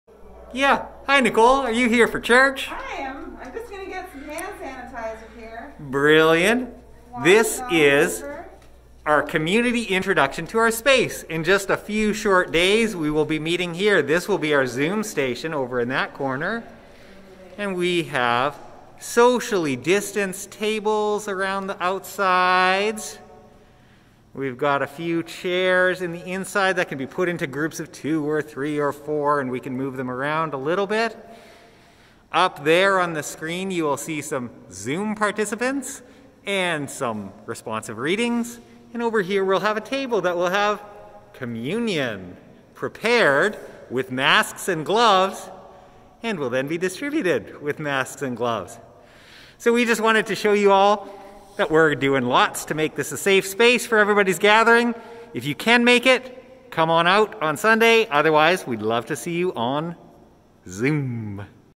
The Road Church Fall 2020 ReLaunch!
A quick look at some of the new Covid-19 protocols that we are putting in place for this weekend to ensure we all stay safe and healthy! Hope to see you there!